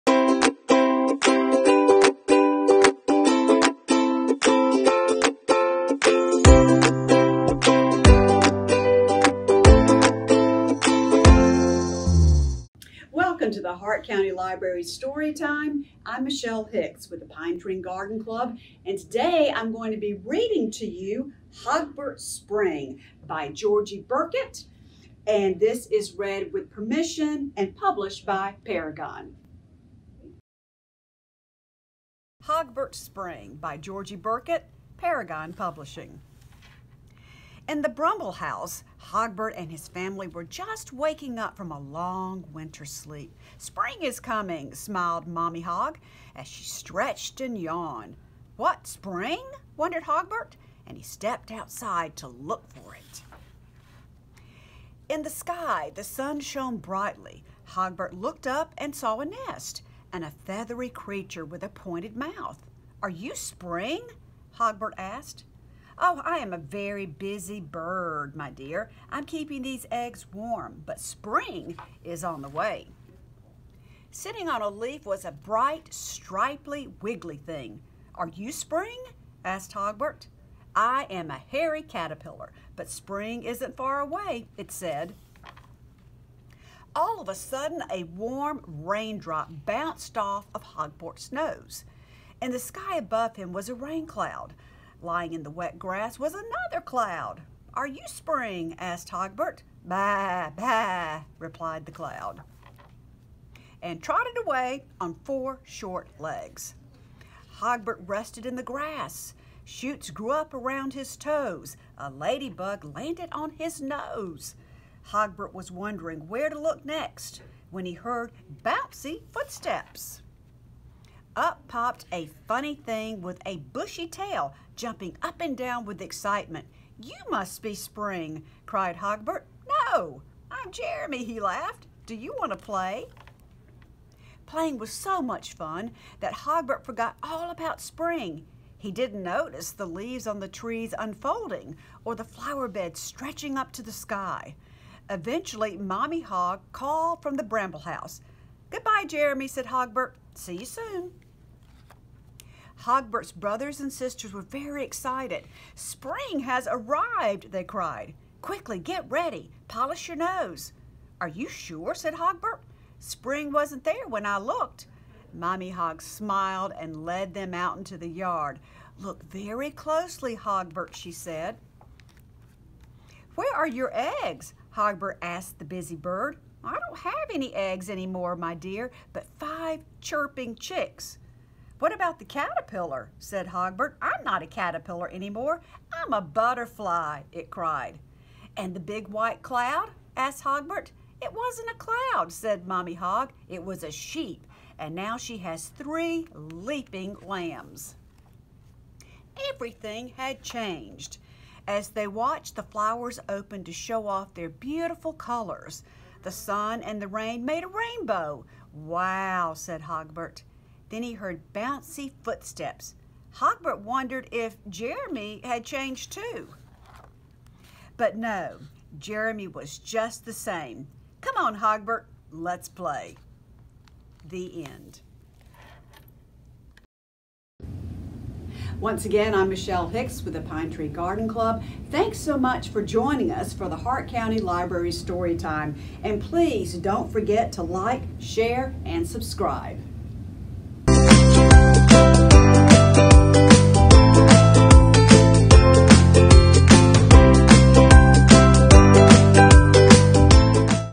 Read Aloud Storytime With The Pine Tree Garden Club
We hope you enjoy this special Storytime! Joining us today is Michelle Hicks from The Pine Tree Garden Club. She reads the adoraible story, "Hogbert's Spring." Written by Georgie Birkett and published and read with permission by Parragon.